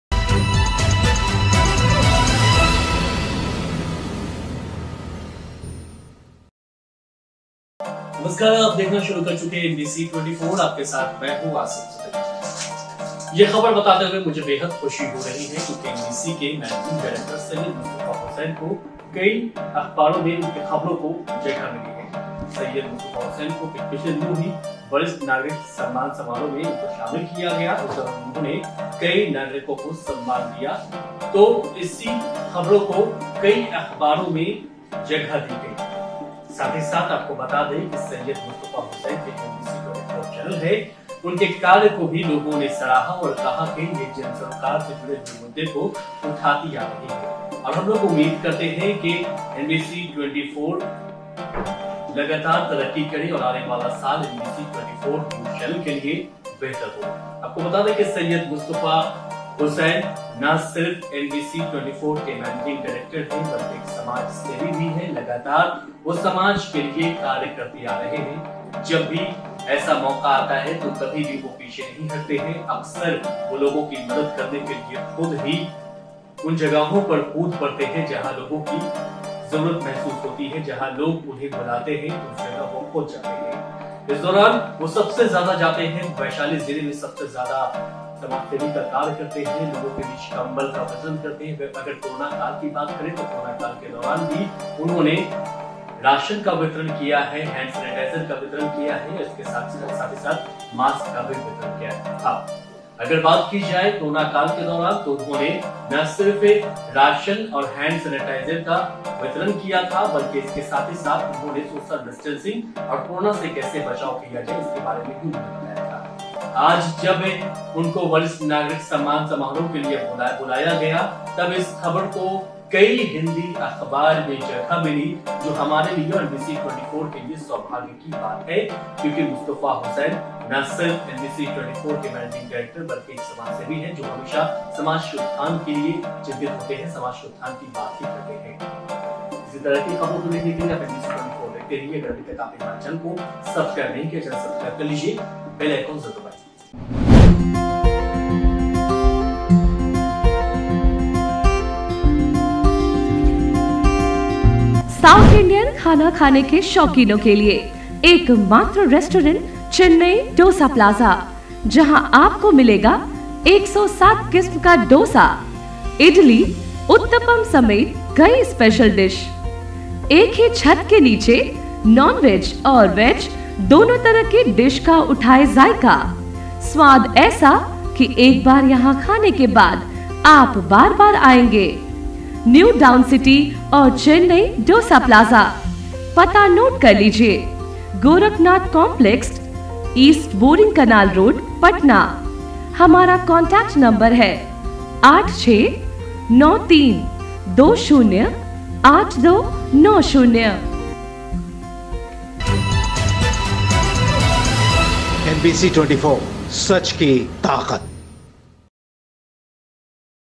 NBC 24 के MANAGING DIRECTOR सैयद मुस्तफा हुसैन के कार्यों को लोगों ने सराहा, हर दिल अजीज बने..
NBC 24 के MANAGING DIRECTOR सैयद मुस्तफा हुसैन के कर्यों को लोगों ने सराहा, हर दिल अजीज बने..